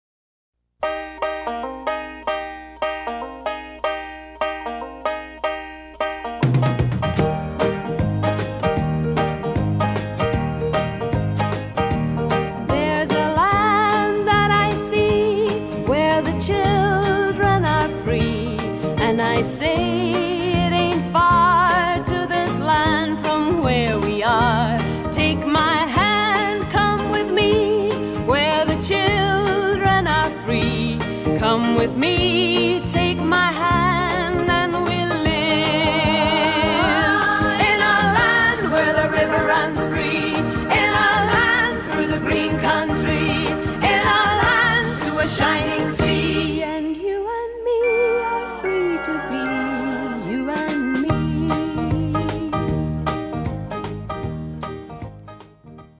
Free To Be... Ruby
My dog Ruby is quite joyful.